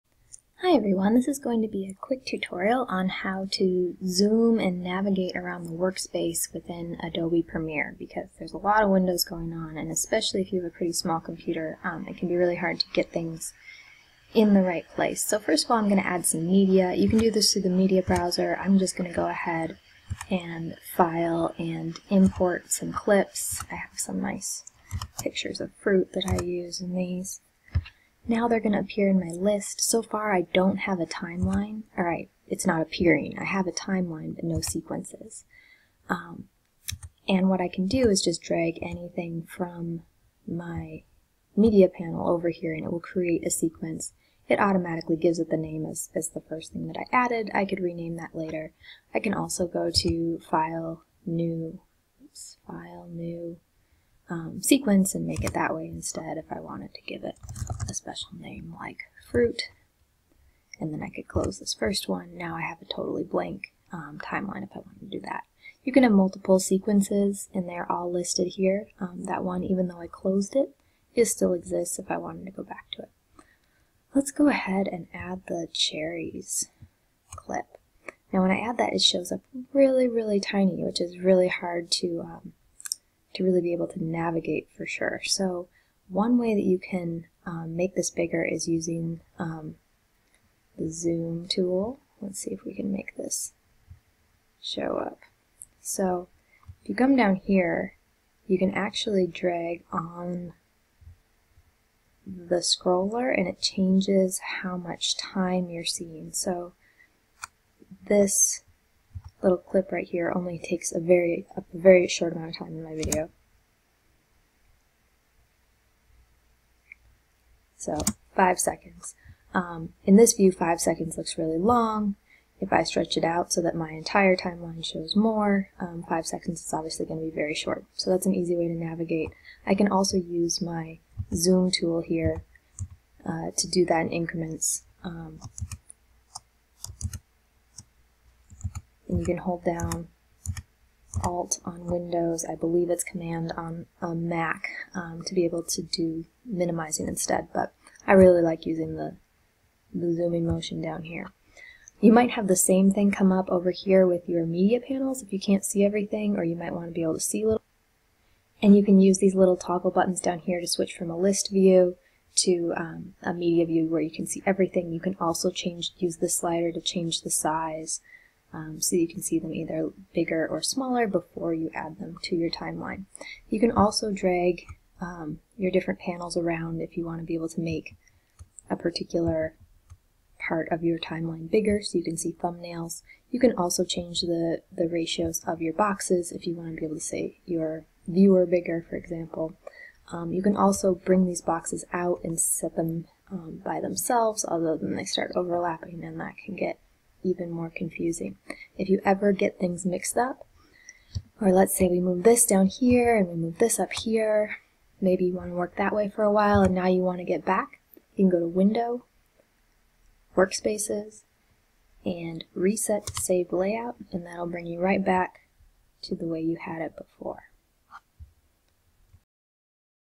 Premiere CC zooming and workspace tour
Tips on how to change the way the workspace is arranged in Adobe Premiere, particularly how to zoom in and out on the timeline and expand clips vertically to more easily edit them.